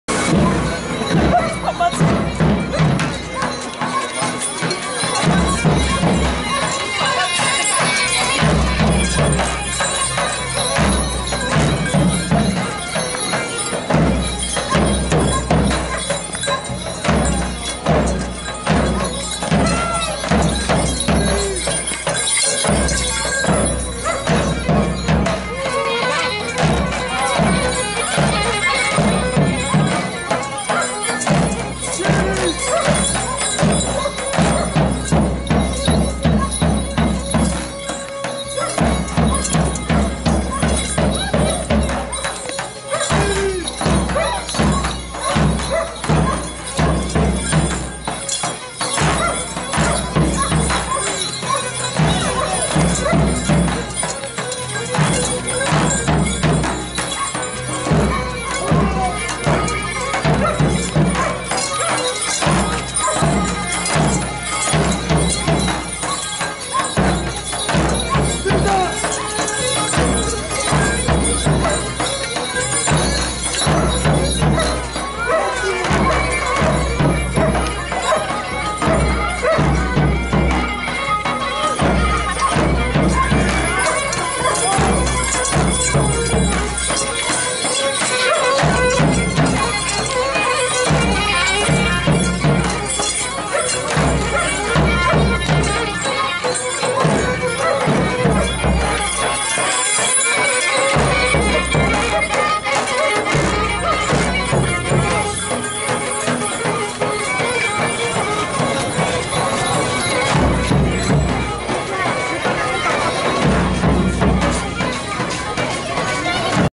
Μωμόγεροι Σύλλογος Ποντίων Ευόσμου «Παναγία Κρεμαστή» 2019
Οι Μωμόγεροι χορεύουν στη διασταύρωση των οδών Αριστοτέλους και Καραολή Δημητρίου